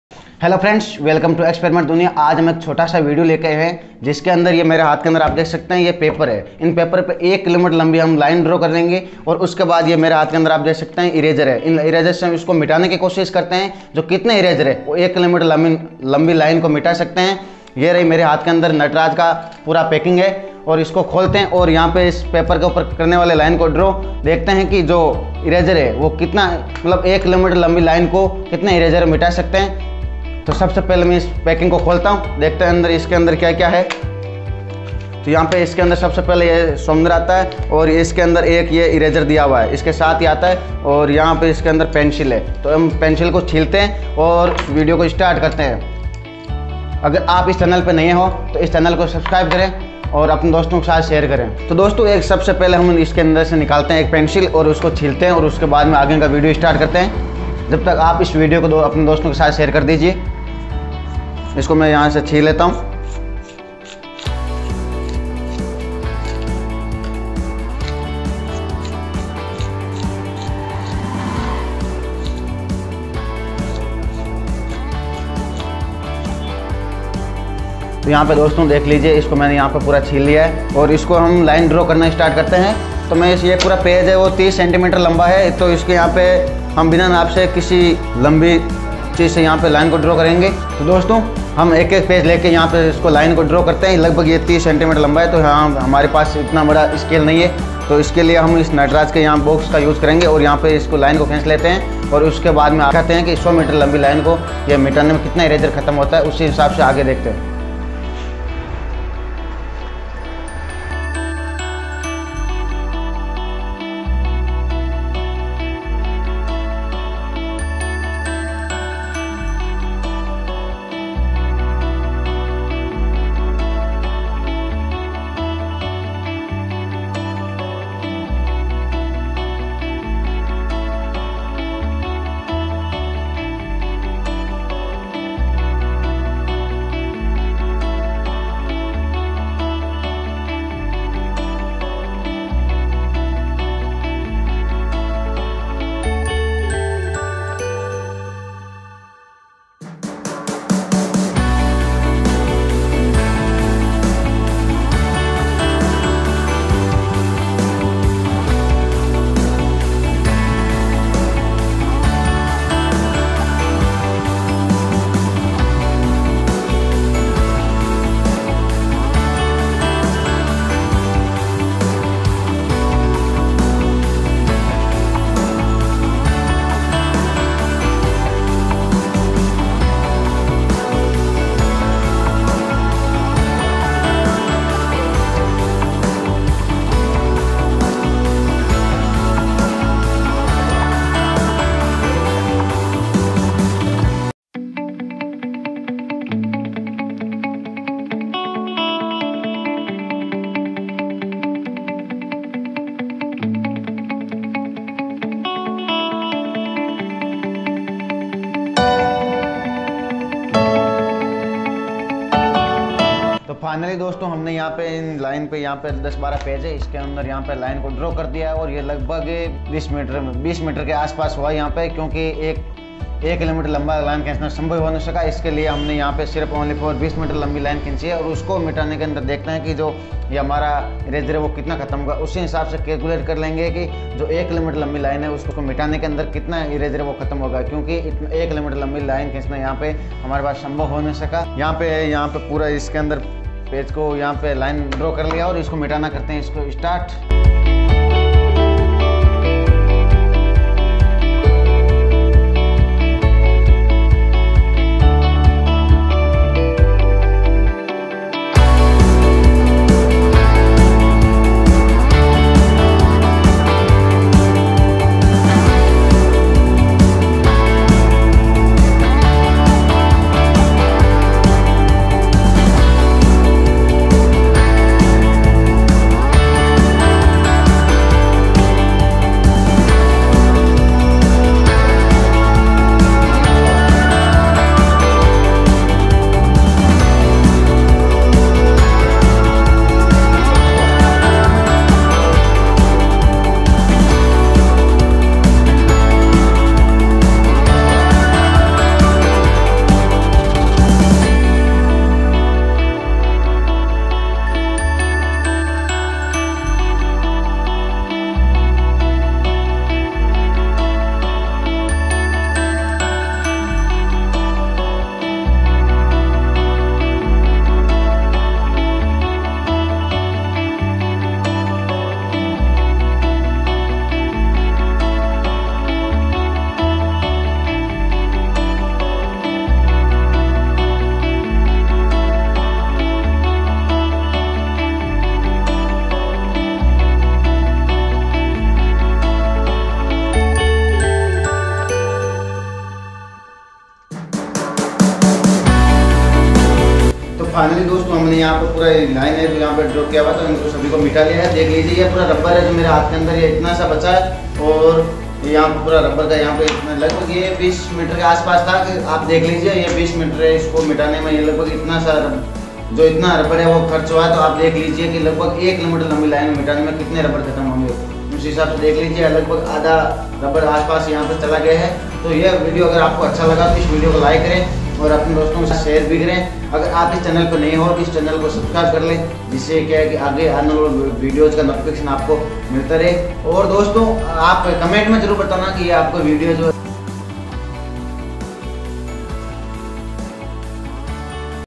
एक किलोमीटर लंबी लाइन मिटाने में कितना रबड़ खत्म होगा || Eraser Rubbing Range Test || Mr Duniya
एक किलोमीटर लंबी लाइन मिटाने में कितना रबड़ खत्म होगा ||Eraser Rubbing Range Test || Experiment Duniya